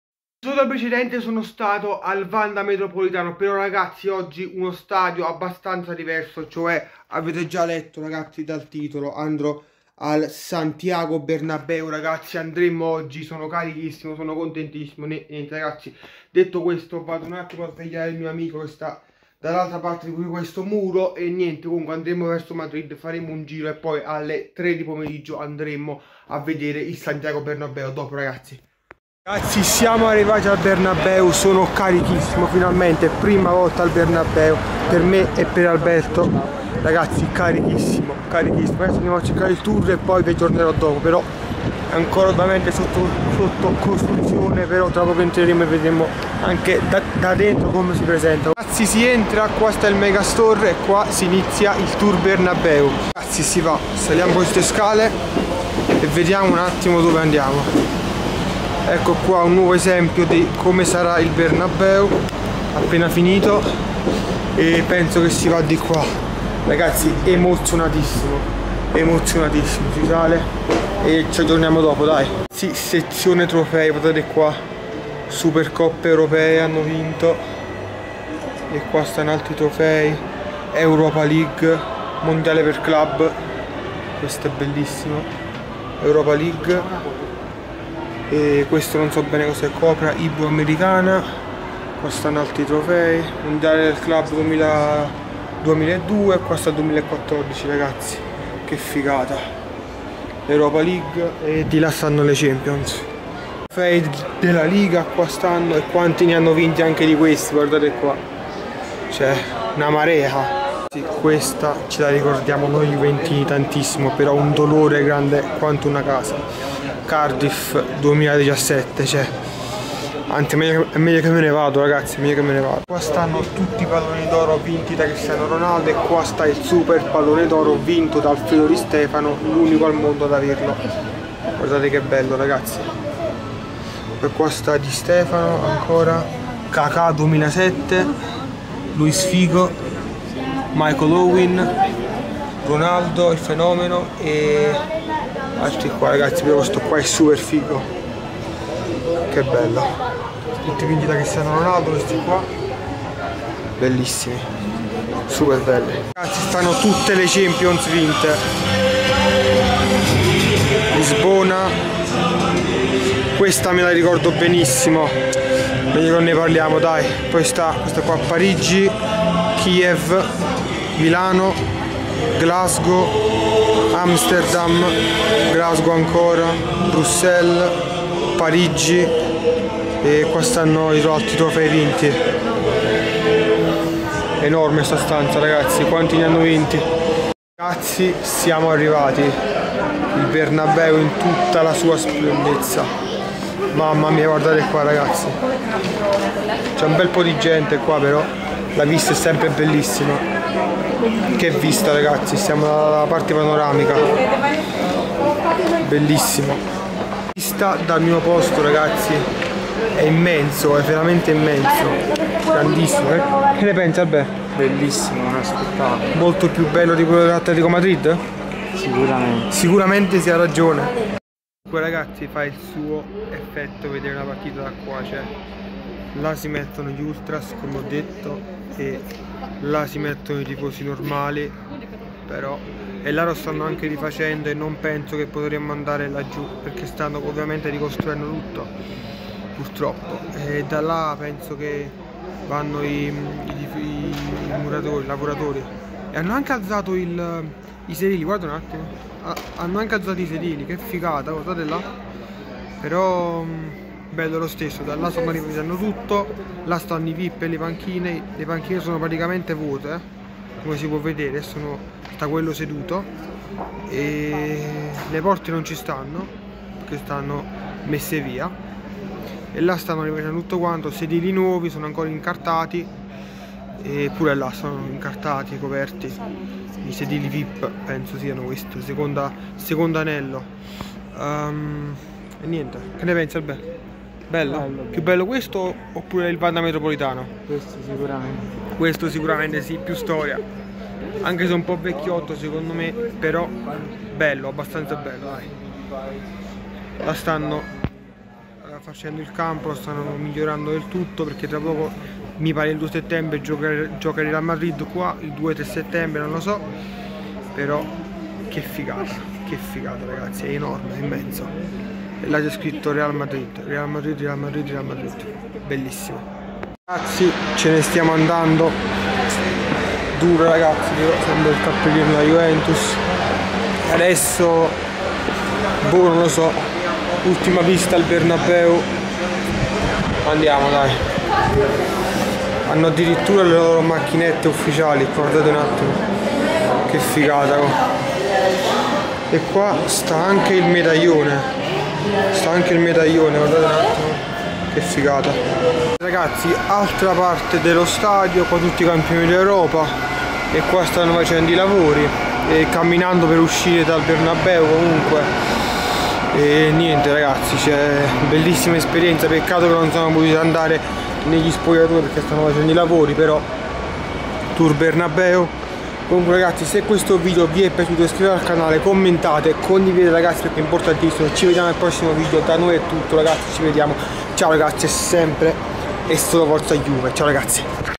Sono stato allo Stadio del Real Madrid - Santiago Bernabeu Tour.
Dopo la Visita allo Stadio del Atletico Madrid a grande richiesta sono andato anche allo stadio del Real Madrid il Santiago Bernabeu.
Questo video e un po' corto ma per il semplice motivo che cera ben poco da vedere seno ovviamente sarebbe stato piu lungo.
comunque se volete altri video come questo fatemi sapere nei commenti dove dovrei andare nel prossimo vlog.
Grazie raga e sempre forza Juve.